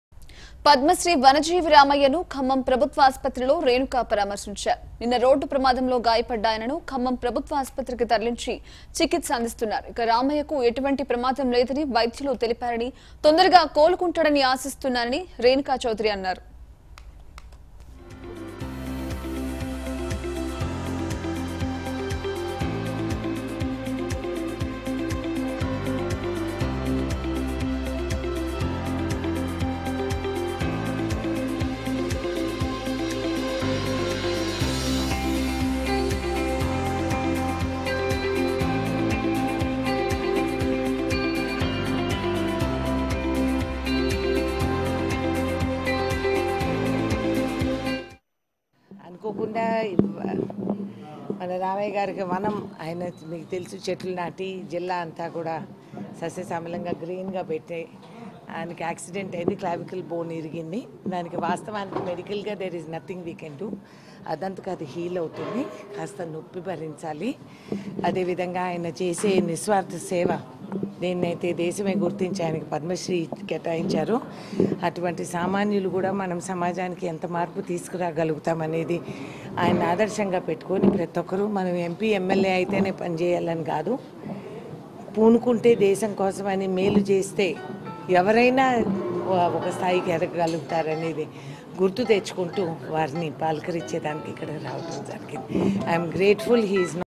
Renuka Chowdhury Pays Visit To Padmasri Vanajeevi Ramaiah At Khammam Govt Hospital
Renuka Chowdhury Pays Visit To Padmasri Vanajeevi Ramaiah At Khammam Govt Hospital llBharat Today